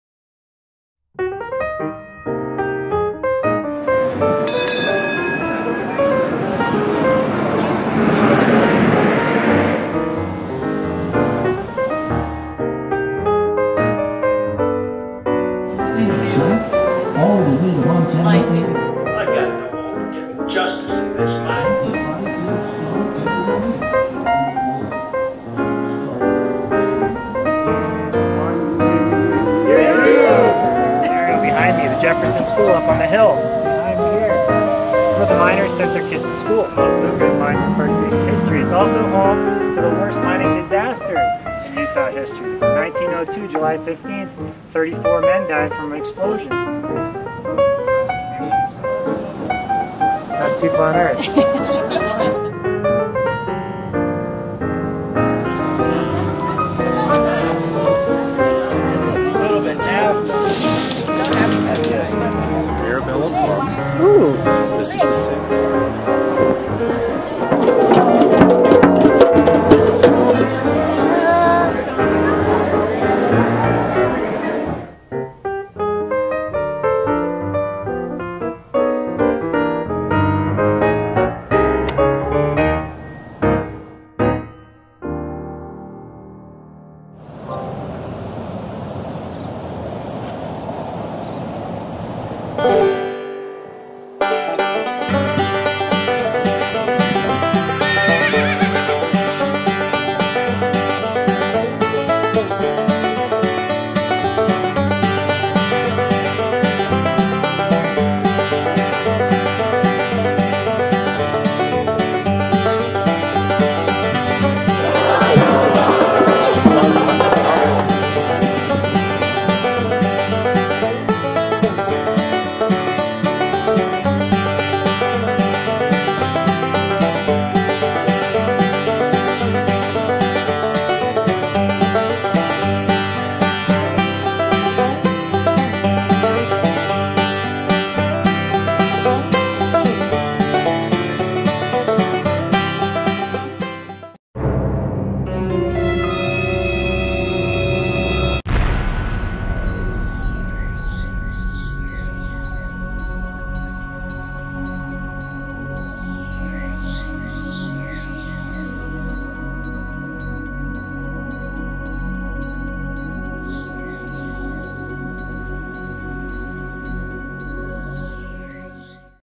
Fall in Park City, Utah - Park City Chamber/Bureau
There's no better time to experience Park City than in the fall; incredible colors, warm days, cool nights, with golfing, hiking, biking, and shopping at its best.  for information on Park City's events, activities and attractions.

Photo and Video by:  Vicki Gaebe
Interactive Manager
Park City Chamber/Bureau